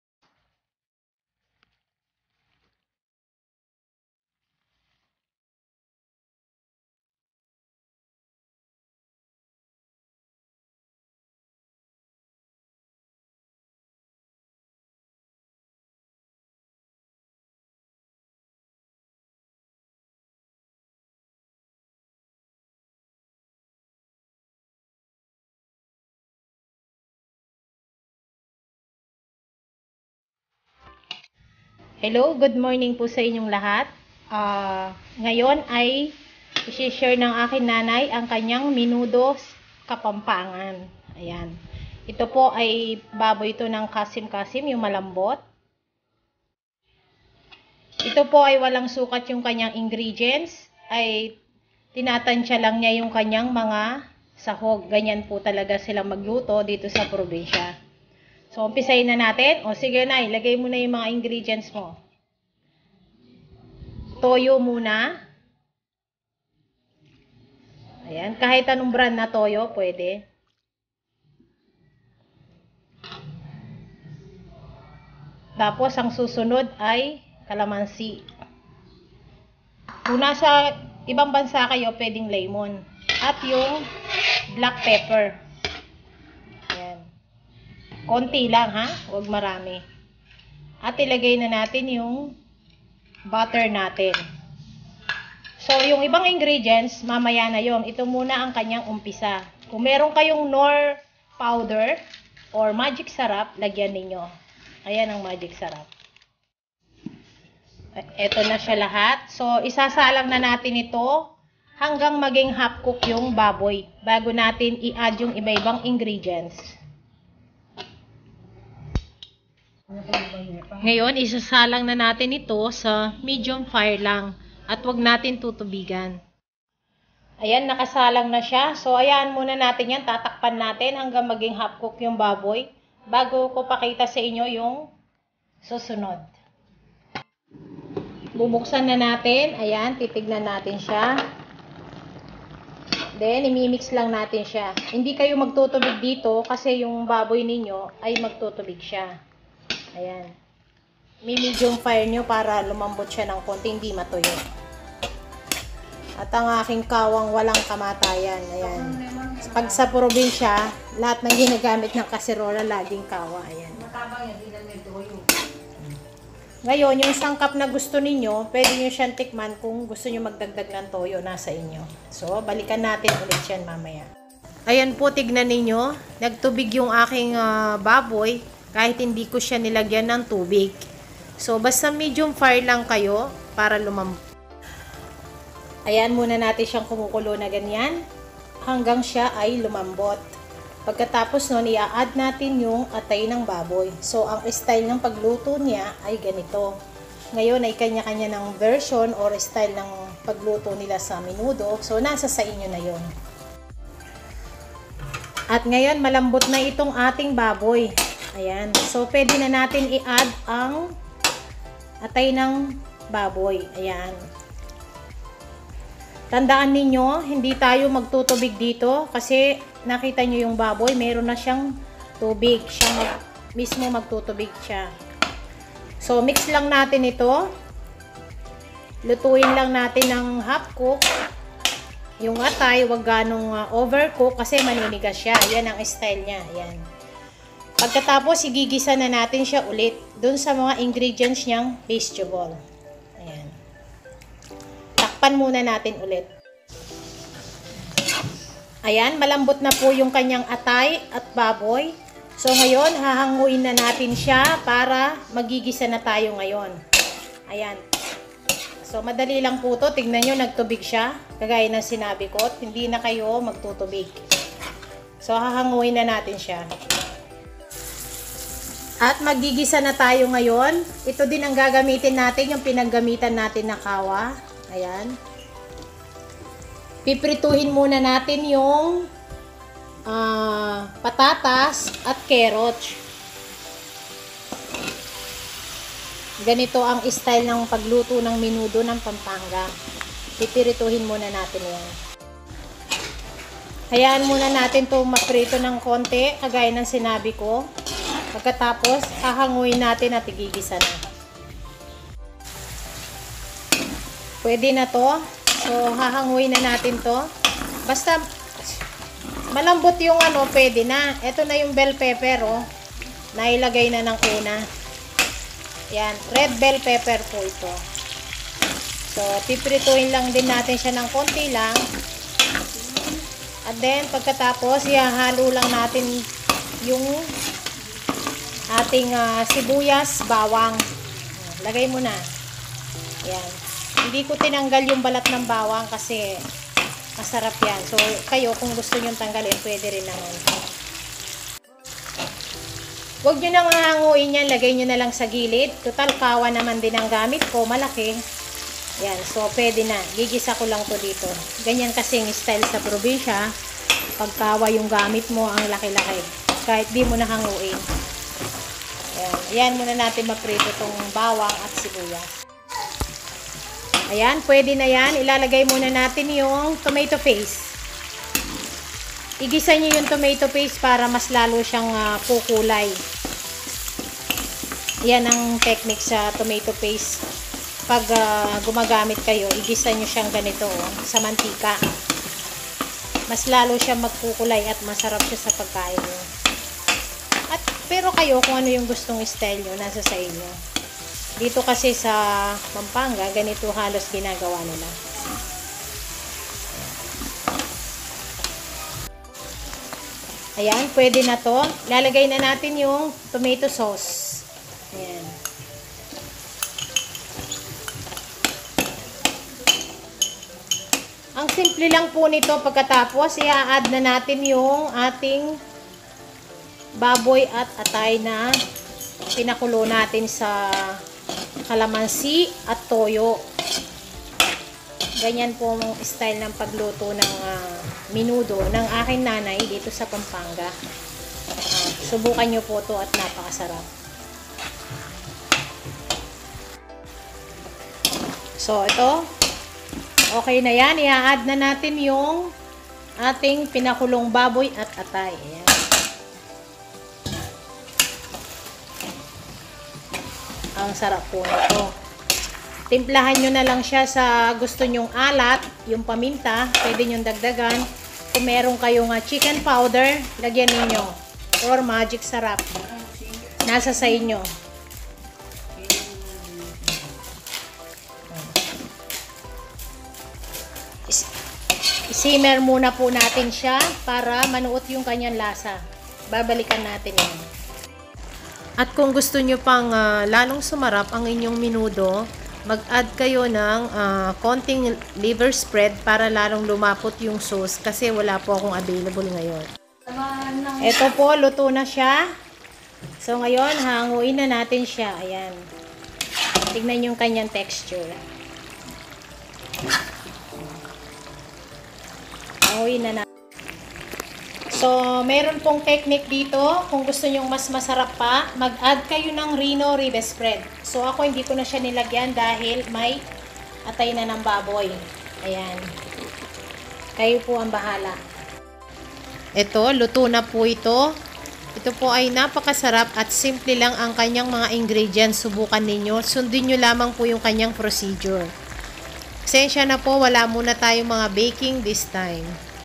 VACATION 12 NANAY'S PORK MENUDO SPECIAL NG PAMPANGA/ PORK MENUDO
Ingredients
First step
Kasim kasim pork
Lemon
Toyo
Black pepper
Magic sarap
Butter
Pork liver

Second step
Oil
Potatoes
Carrots
Red bell pepper
Garlic
Onions
Tomato paste
Tomato sauce
Black pepper to tease
Reno liver spread
( kung gusto mag add ng hotdog puwede )

Procedure
Sa first step pakuluin muna ang baboy sa low fire at wag mag add ng water , dahil mag tutubig ang pork
Then pag malambot na pork add na ang pork liver
Don’t over cook
Pag malambot na sila
Set aside

Pamunta ba sa second step

Sa second step
Prituhin muna ang carrots at patatas at red bell pepper
Pag malambot na carrots and patatas, set aside
Isunod na bawang sibuyas
Then tomato paste tomato sauce igisa eto kasabay bawang sibuyas
Then ihalo na ang mixture na naluto na pork and liver
Shimmer nalang eto angang maluto

Sundin nalamang po procedure sa video